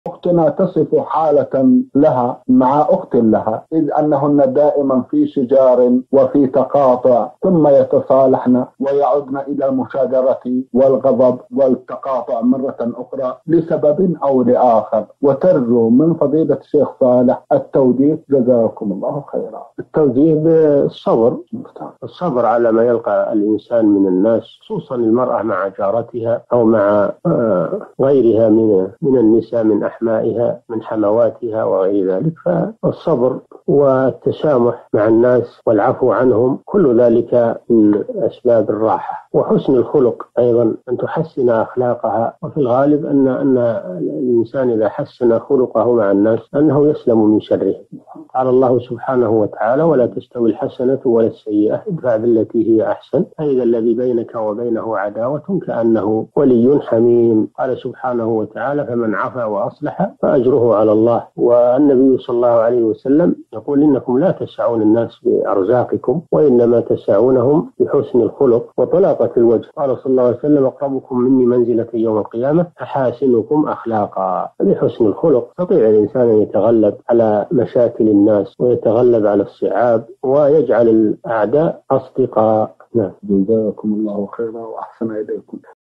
فتاة في حالة دائمة من الشجار والتقاطع مع أخت لها ثم يتصالحن و يعدن للشجار فما توجيهكم؟ الشيخ الفوزان
نص السؤال:
أختنا تصف حالة لها مع أخت لها إذ أنهن دائماً في شجار وفي تقاطع ثم يتصالحن ويعدن إلى المشاجرة والغضب والتقاطع مرة أخرى لسبب أو لآخر وترجو من فضيلة الشيخ صالح التوجيه جزاكم الله خيراً؟
السيرة الذاتية :
للاشتراك في قناة الشيخ صالح الفوزان

رئيس الفريق
لمشروع كبار العلماء